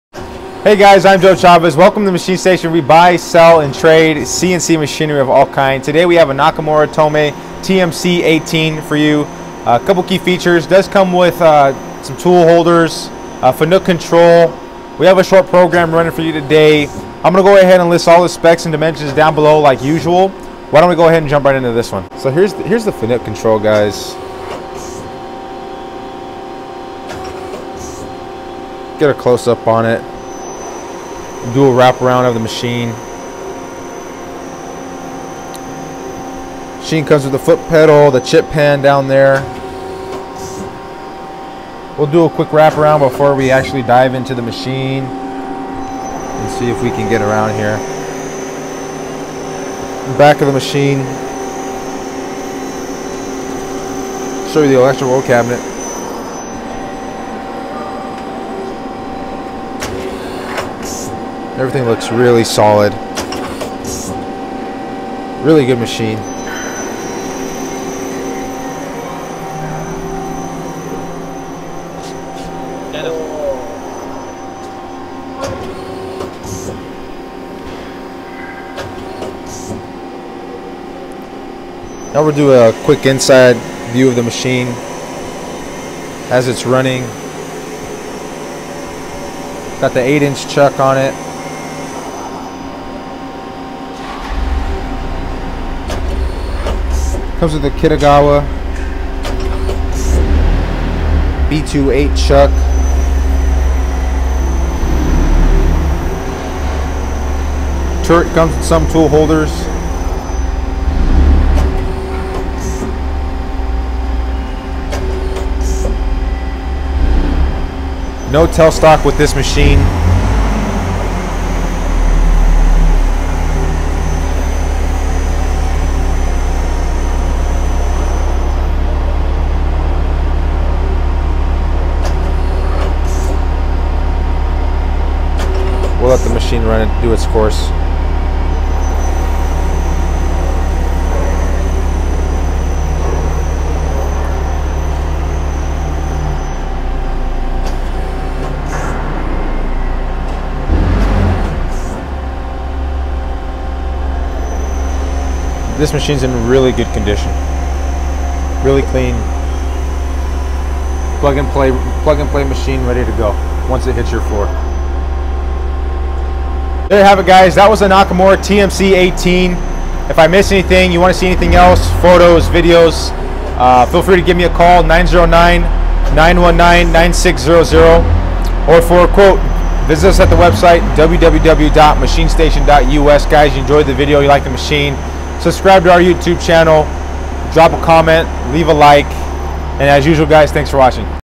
Nakamura TMC-18 CNC Turning Center Lathe
Make: Nakamura
Model No.: TMC-18
Serial No.: A182706
Condition: Very Good

“This machine is in good running condition. It is equipped with 8″ ChucK. On Our Floor fully powered / tested & is in Great working condition."

NAKAMURA TMC-18 CNC  Equipped With:

Fanuc 21T CNC Control
8" 3 Jaw Hydraulic Chuck
A2-6 Spindle
Coolant System
Barfeed Interface
RS-232 port
10 Station Automatic Turret
Foot Switch
Manuals

NAKAMURA TMC-18 CNC  Specifications:

Capacity:
Max Turning Diameter: 11.02"
Max Turning Length: 11.22"
Max Bar Capacity: 2.05"
Swing Over Bed: 15.75"

X Axis Travel: 6.3"
Z Axis Travel: 12.5"
Rapid Traverse X Axis: 630 IPM
Rapid Traverse Z Axis: 787 IPM

Spindle Nose: A2-6
Spindle Bore: 2.3"
Spindle Speed: 0 - 4500 RPM
Spindle Motor: 15 HP

Turret Capacity: 10 Stations
Turret Index Time: 1 Second

General:
Power:  220v / 3-Phase
Overall Dimensions: 70" x 66" x 70" High
Weight: 6000 lbs

NAKAMURA TMC-18 CNC  Comes as shown in pictures..